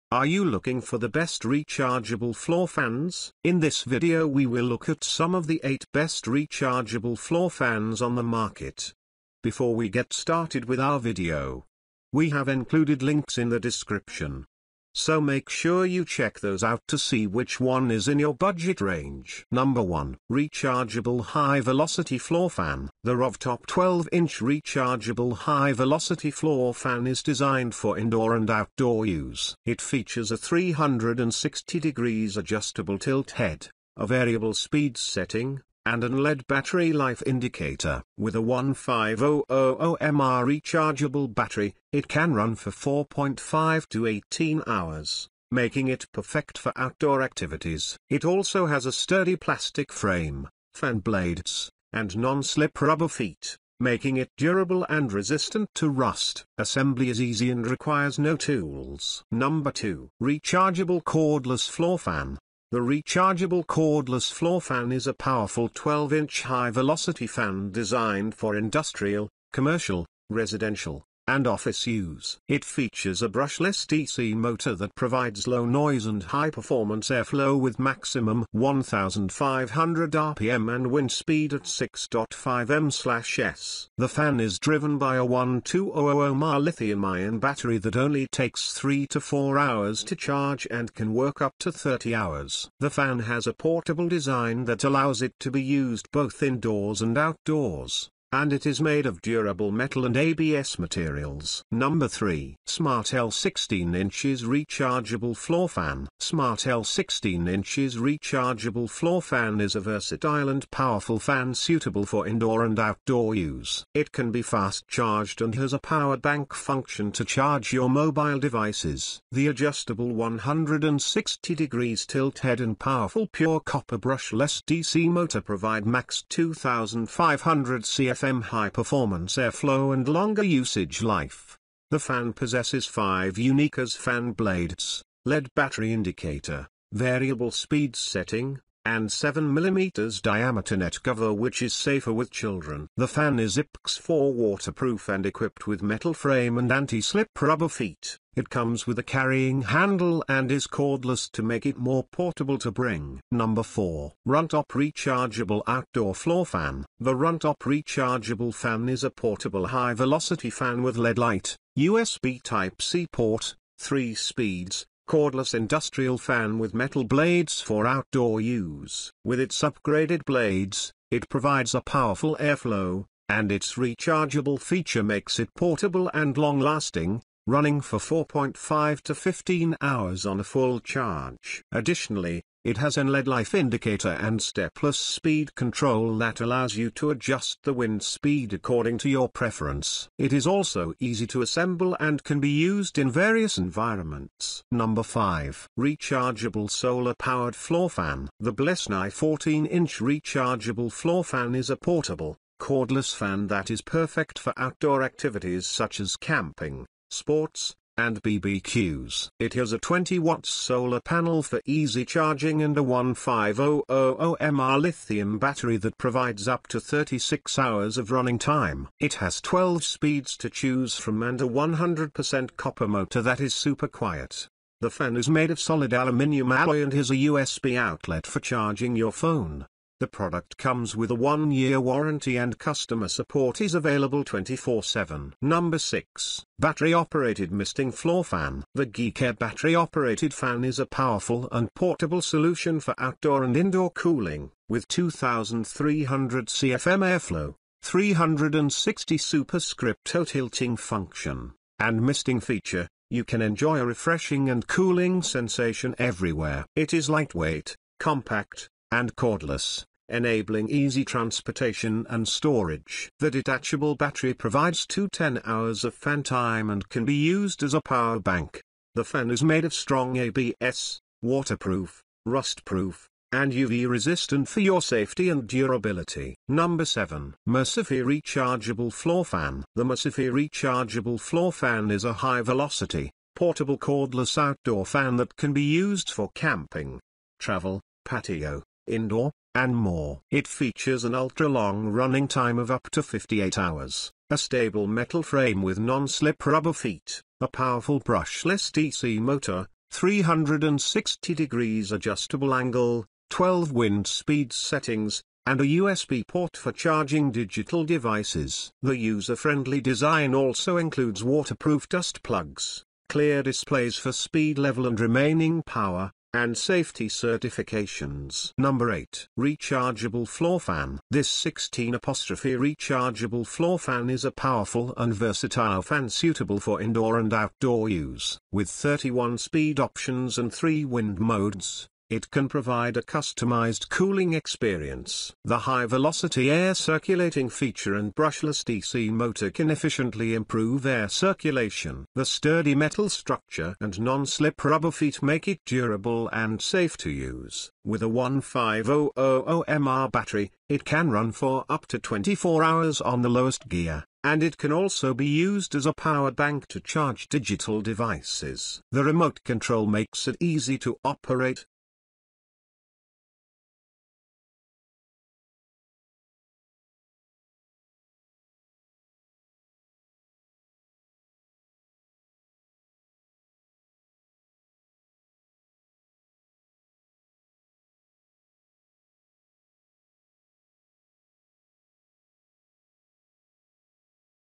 ✅TOP 8 Best Rechargeable Floor Fans in 2023
1. Rechargeable High Velocity Floor Fan.

2. Rechargeable Cordless Floor Fan.

3. Smartele 16" Rechargeable Floor Fan.

 4. RUNTOP Rechargeable Outdoor Floor Fan.

5. Rechargeable Solar-Powered Floor Fan.

6. Battery Operated Misting Floor Fan.

7. MOCIFI Rechargeable Floor Fan.

8. Rechargeable Floor Fan.

Best smartphone
Best camera smartphone
Best 5G smartphone
Best Gaming smartphon